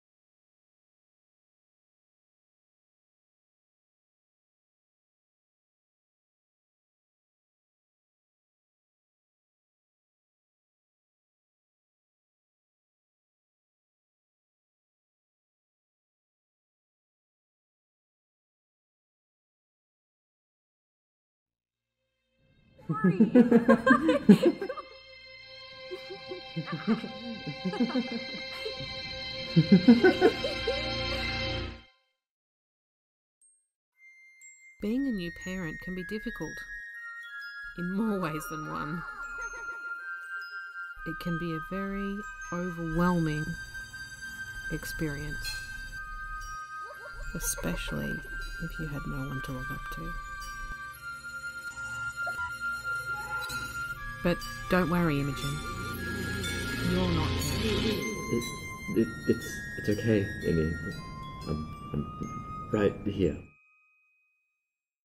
Troubles, My Dear - Trailer (school project) [disclaimers]
Being a new parent can be difficult, in more ways than one
It can be a very overwhelming experience, especially if you had no one to look up to

A first-time mother adopts a young child, but will she be able to raise them properly with the lack of knowledge on how to be a proper mother?

AAHHH I HOPE YOU LIKE MY MEDIA PROJECT FOR THIS SEMESTER IM PRETTY PROUD OF IT!
yes I am releasing this on my bday I cannot wait ANY LONGER

I’m 17 now btw yippee
u may send ur birthday wishes in the comments if you’d like !
i did a lot today so i didnt get time to complete a birthday animation haha

I hope you enjoyed this trailer tho!!! I’ve spent a few months on this !!! :]

Troubles, My Dear - a story created by ​⁠​⁠@mangoanimationsxd and ​⁠​⁠@xenomet

THUMBNAIL BY SAVIOURCEREAL AS WELL SHES SO GOOD AT ART

Voice actors:
- my Mum (throughout)
- me (background mainly)
- Delmarve (start and end)
- many others (jumble at the end)

Started: March
Finished: June
Fps: 12
Program: FlipaClip

expect more tmd content in the future!!! I have been WAITING SO LONG TO MAKE STUFF ABOUT IT!
if you wanna learn more about tmd, Cereal and I are working on some stuff for that!!! in our own time!!! it may take a while LMAOOOO so be patient!!

shares would be greatly appreciated btw,, let me know if you’re interested in the story, because there’s so much more to it than this,,,,

love u all mwammwmamwma see you soon! tysm for watching!!